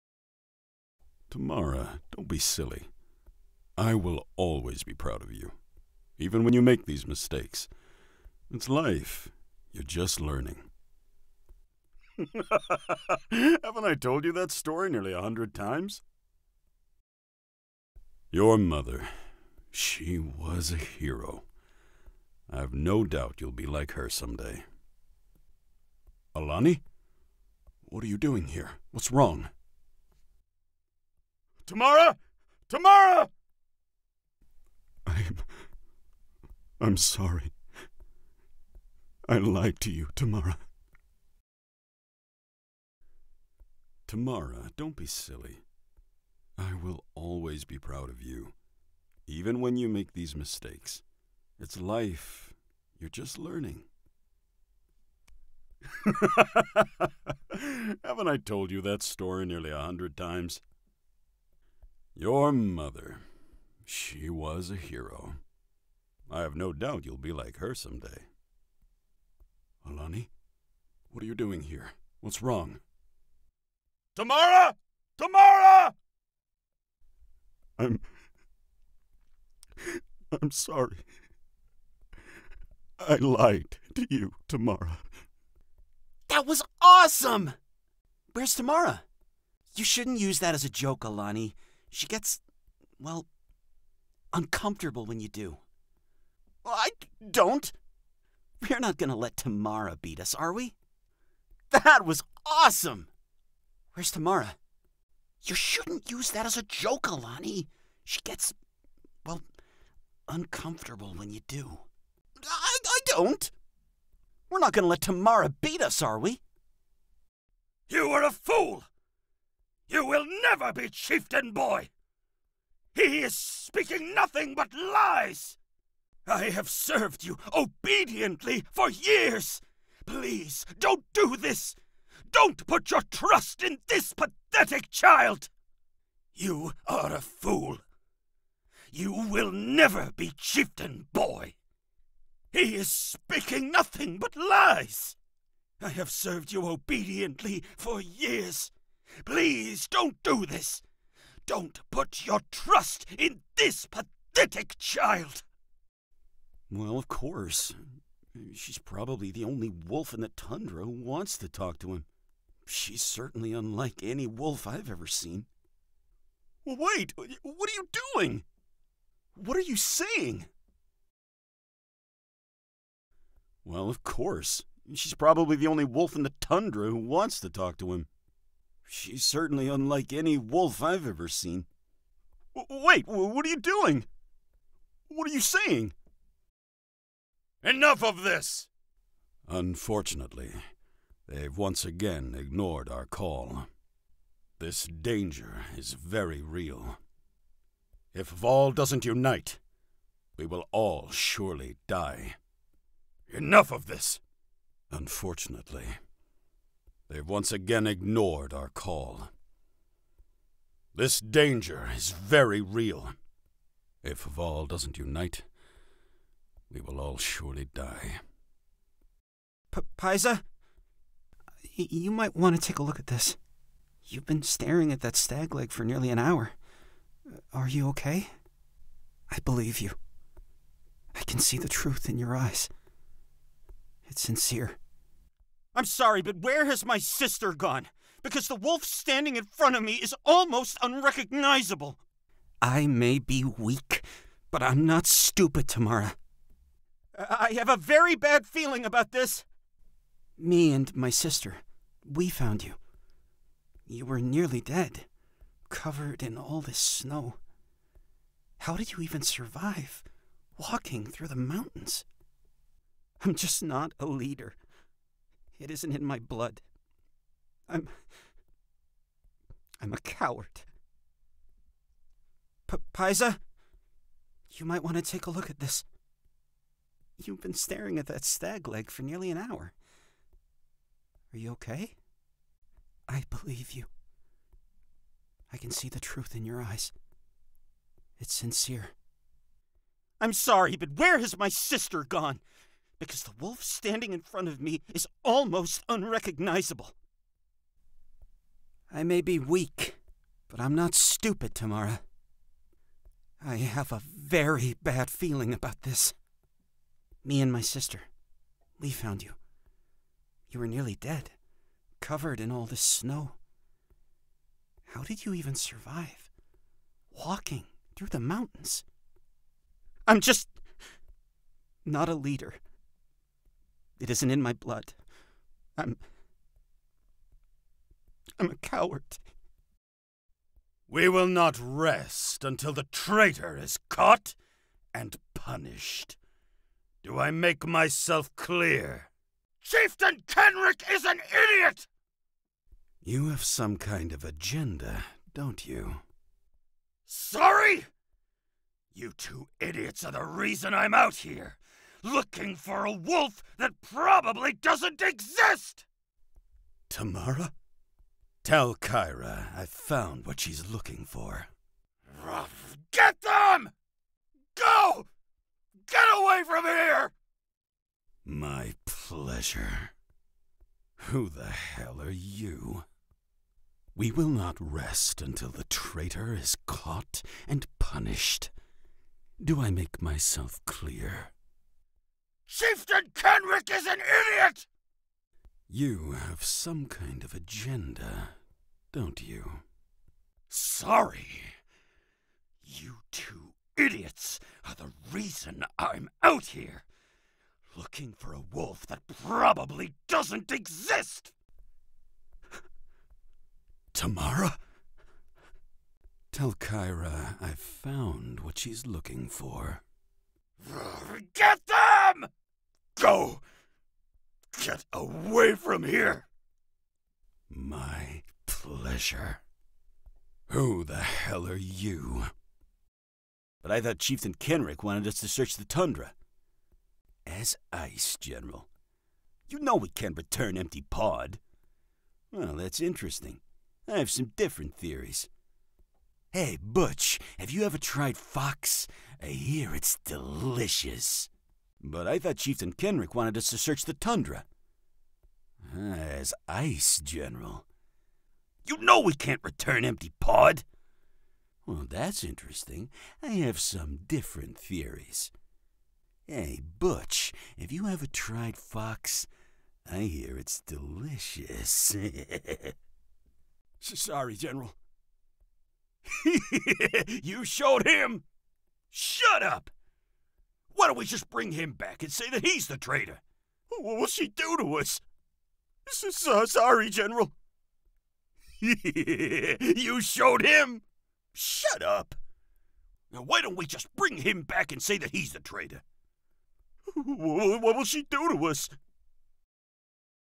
Phillip Auditions: Wolf Moon
This video features auditions for ForestPanic's upcoming animated series, Wolf Moon (more info on the casting call can be found